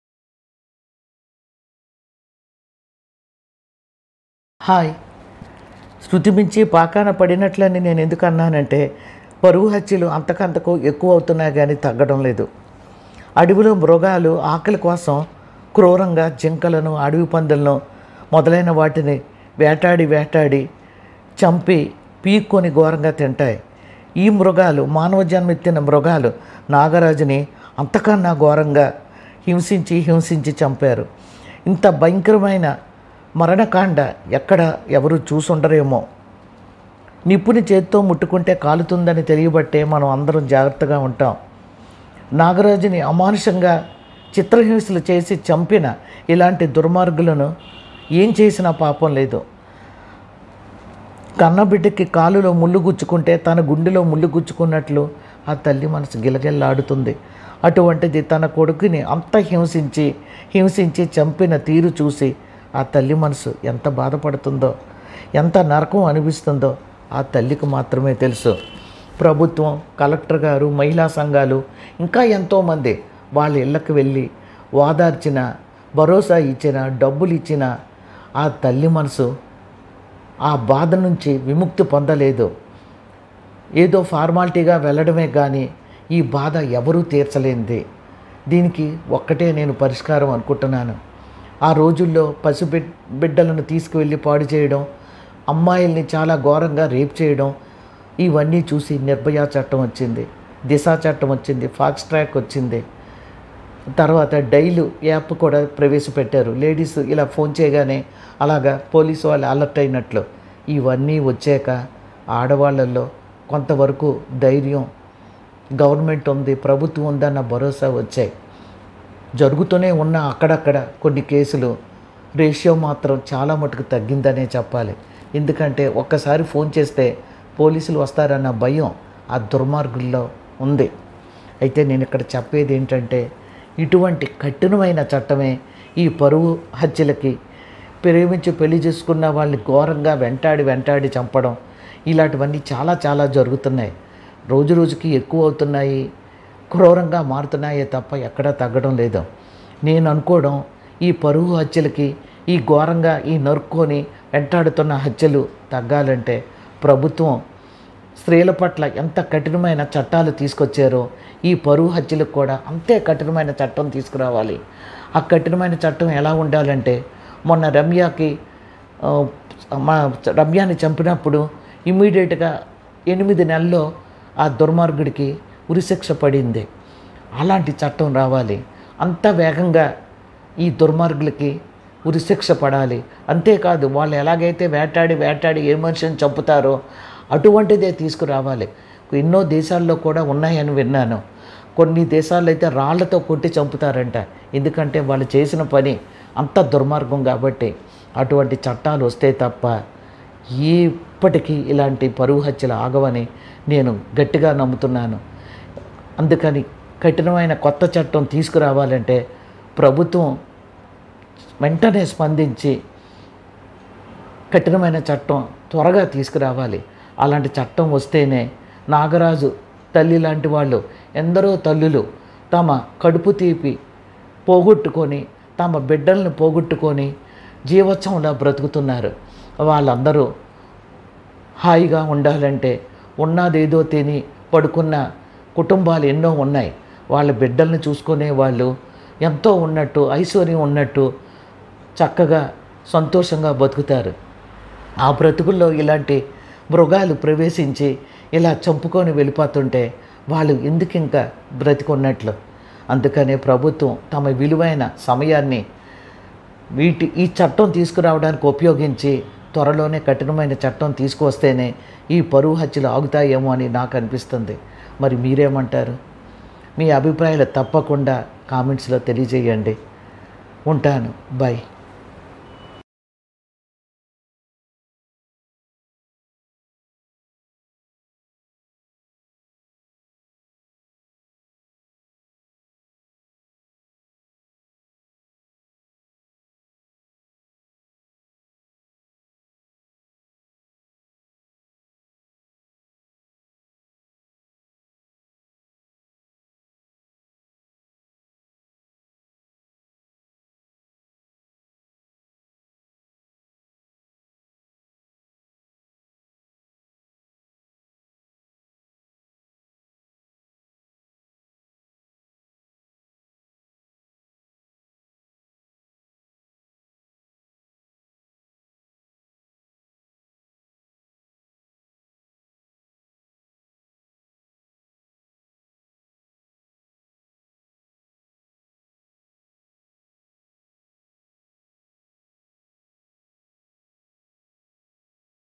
ఈపరువు హత్యలు ఆగేదెప్పుడు ?
పరువు హత్యలు    రోజు  రోజు పెరుగుతున్నయి తప్ప తగ్గడం లేదు .ప్రభుత్వం కఠినమయిన చట్టం తీసుకు రావడం  శిక్ష వెంటనే అమలు చేయడం తప్ప వేరే మార్గము  కనబడటం లేదుఘోరమయిన, క్రూరమయిన దుర్మరుగులు  ఏ విధంగా నాగరాజుని చంపారో  వాళ్ళని ఆలా చంపి నప్పుడే  ఇలాంటి హత్యలు చేసే వెధవలికి  భయం వస్తుంది .!

paruvu Hatyalu#meethookasepunenu